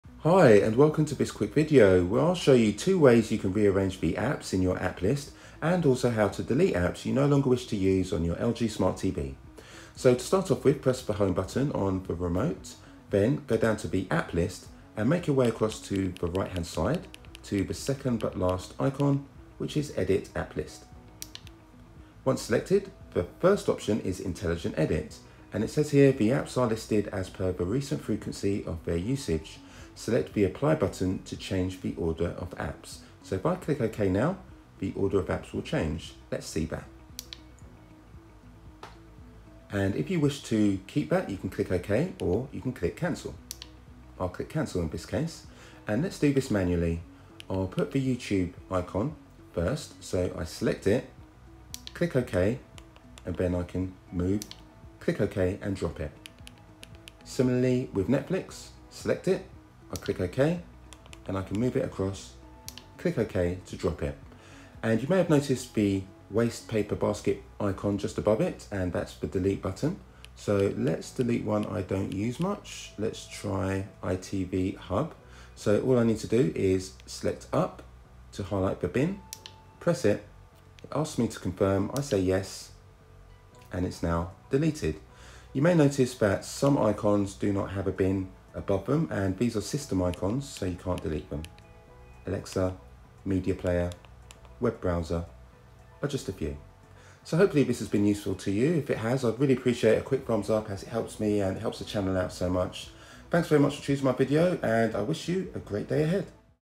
Arrange / Delete Apps *New LG Smart TV
Here's how to arrange and delete apps on the new LG Smart TV. Step by step instructions so you can get your TV ordered the way you want it.
WebOS 6

LG OLED55C1PUB Alexa Built-in C1 Series 55" 4K Smart OLED TV